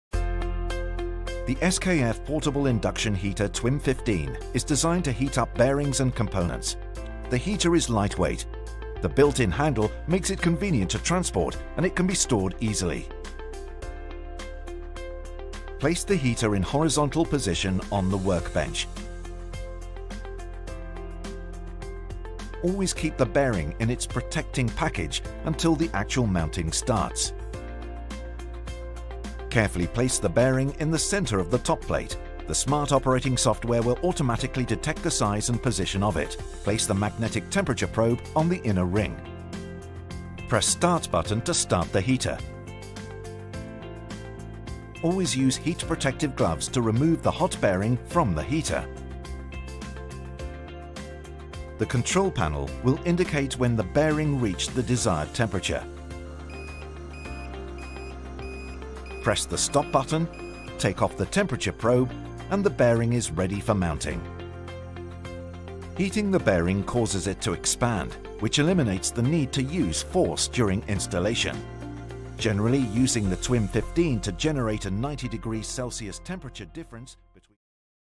Calentador Por Inducción SKF TWIM 15 - Rodamientos Avenida
Calentador de inducción portátil
Portable induction heater TWIM 15

Una solución portátil para calentar rodamientos
El calentador de inducción portátil TWIM 15 de SKF está diseñado para aplicaciones en trabajos de mantenimiento para calentar rodamientos que están montados con un ajuste de interferencia en un eje. El calentamiento del rodamiento provoca su dilatación, lo que elimina la necesidad de utilizar la fuerza durante su instalación. Por lo general, el uso del TWIM 15 para generar una diferencia de temperatura de 90 °C (162 °F) entre el rodamiento y el eje es suficiente para permitir la instalación. Además, el TWIM 15 puede utilizarse para calentar otros componentes metálicos con forma de aro, lo que ofrece flexibilidad de uso.

El TWIM 15, que utiliza energía eléctrica, se caracteriza por su estructura de fibra de vidrio y plástico resistente a altas temperaturas que permite lograr una baja diferencia de temperatura entre los aros interior y exterior del rodamiento. Esto ayuda a reducir las tensiones internas que se generan debido a la excesiva dilatación térmica del aro interior en comparación con el aro exterior.

La unidad presenta un panel de control LED fácil de usar, que no requiere capacitación especial y es fácil de interpretar. El panel se utiliza para regular la temperatura y también indica que el TWIM 15 se encuentra en funcionamiento.